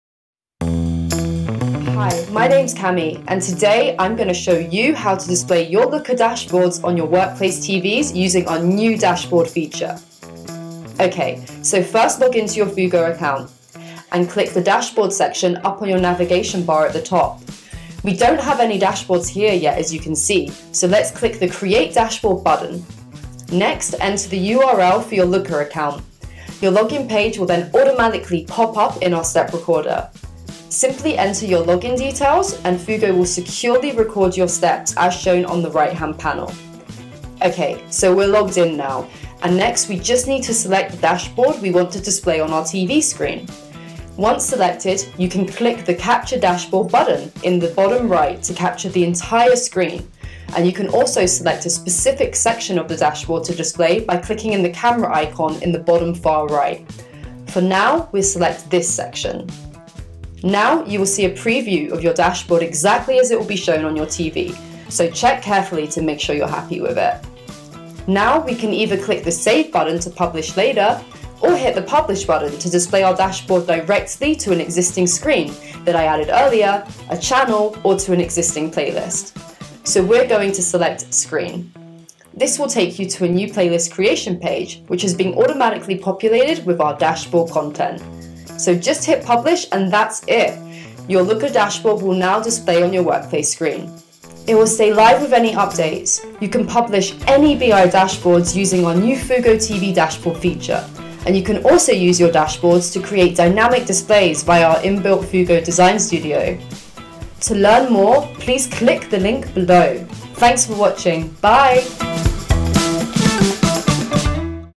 Fugo TV Dashboard: Looker Guide
Learn how to display your Looker BI Dashboard on your digital signage using Fugo TV Dashboard feature.

Resources:
👉Fugo TV Dashboard: